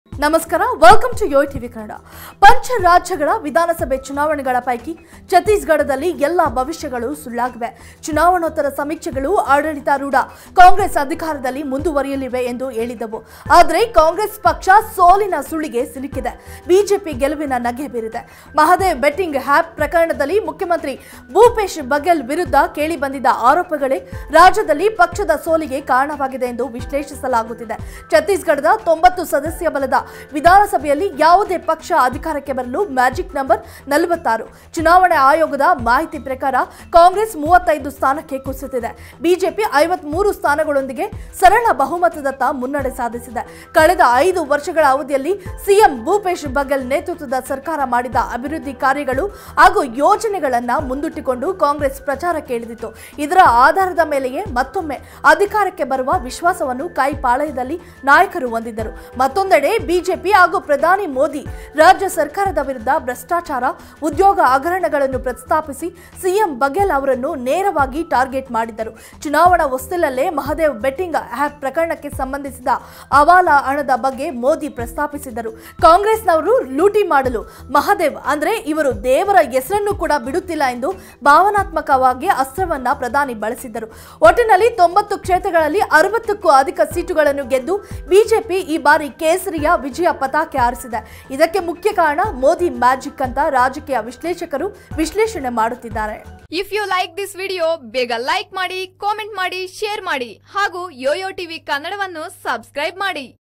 ಮೋದಿ ಮ್ಯಾಜಿಕ್: ಛತ್ತೀಸ್‌ಗಢ ಚುನಾವಣಾ ಫಲಿತಾಂಶ.. ಇಲ್ಲಿದೆ ಸಂಪೂರ್ಣ ಮಾಹಿತಿ.. || Chhattisgarh Election Result
ಮೋದಿ ಮ್ಯಾಜಿಕ್: ಛತ್ತೀಸ್‌ಗಢ ಚುನಾವಣಾ ಫಲಿತಾಂಶ.. ಇಲ್ಲಿದೆ ಸಂಪೂರ್ಣ ಮಾಹಿತಿ.. || Chhattisgarh Election Result | YOYO TV Kannada